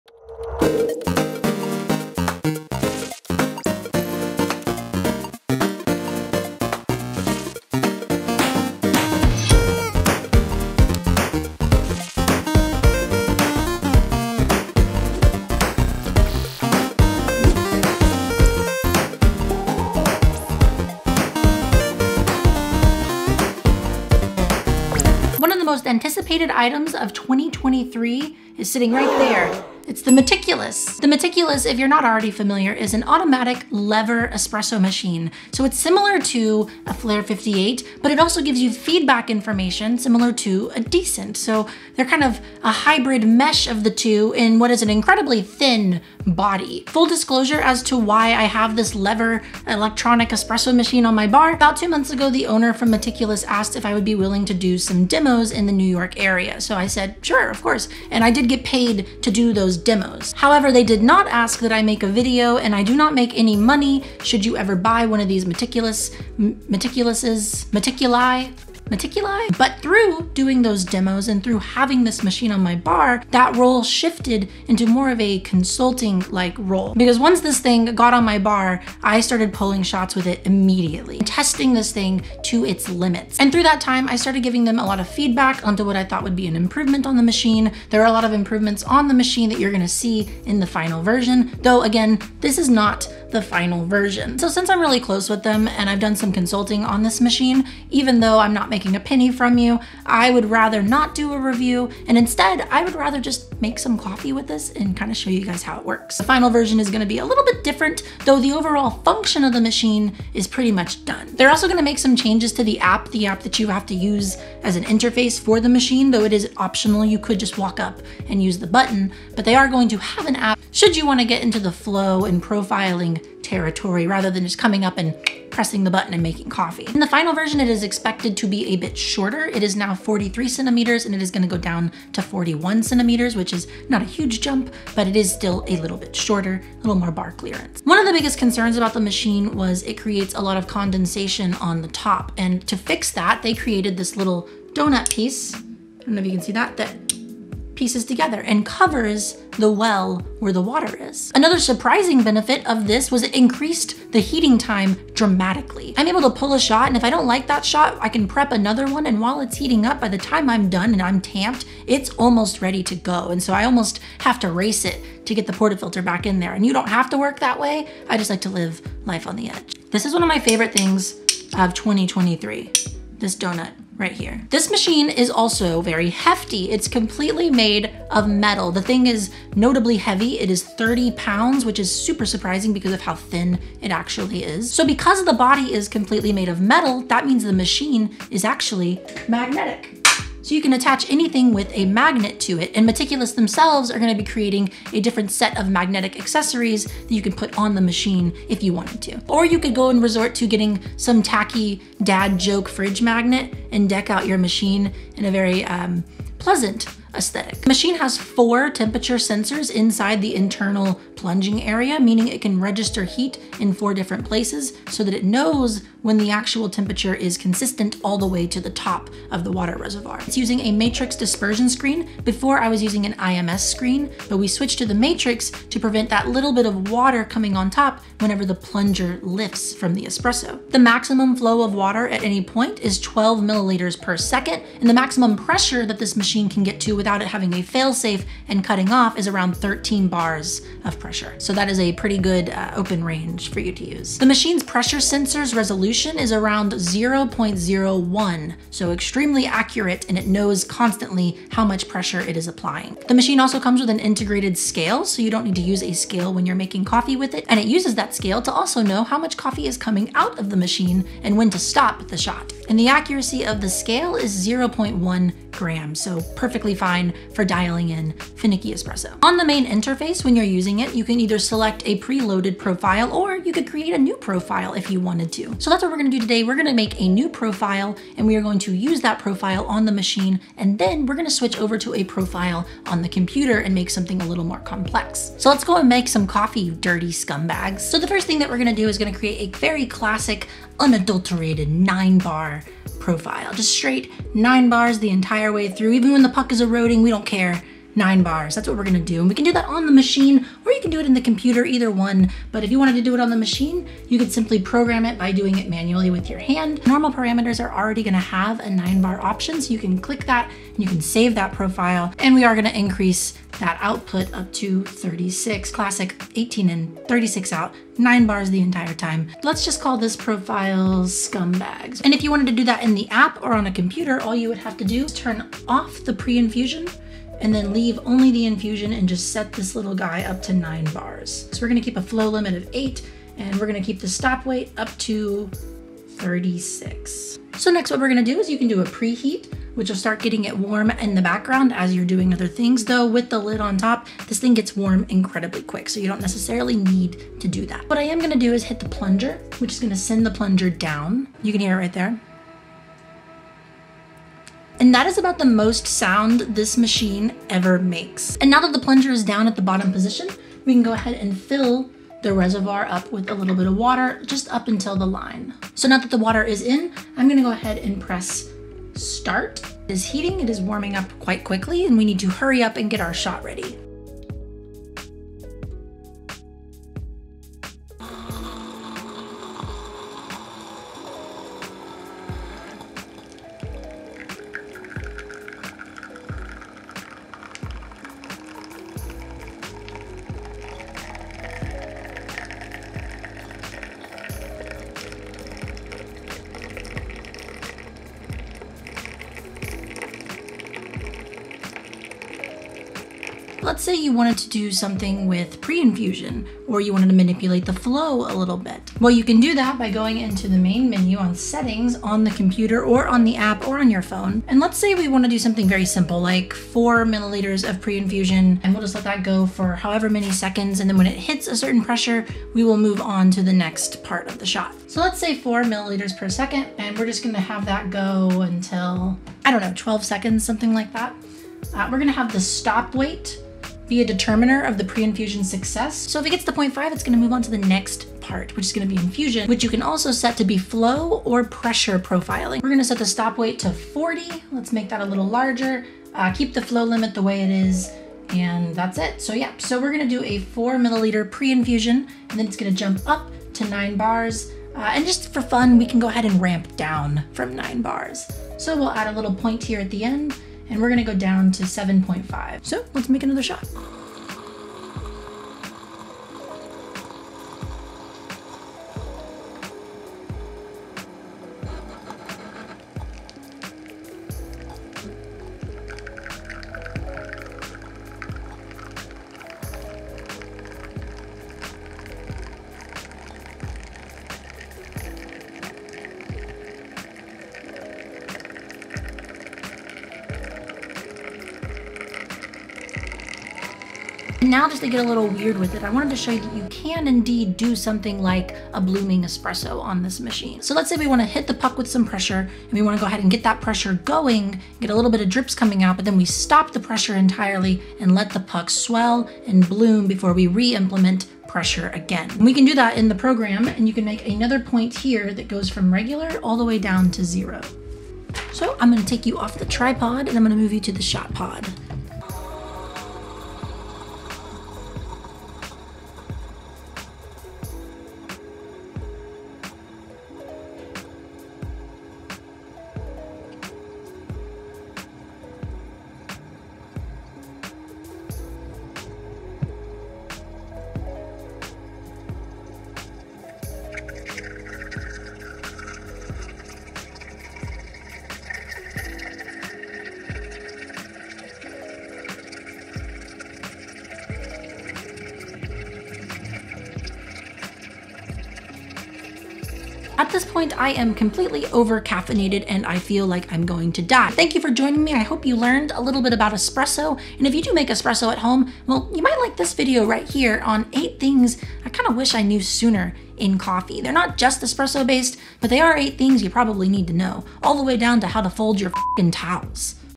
The METICULOUS ESPRESSO MACHINE
Today we breakdown the current version (V8) of the highly anticipated Meticulous Espresso Machine! Along with incoming changes, features and how to make coffee on it.

I will make an update video once I see the final version as well as the second most highly anticipated device... The meticulous steam.

Full Disclosure I have worked with this company, SO this is not a review. But also I am close with them so make sure to form your own independent feelings. All feelings are welcome.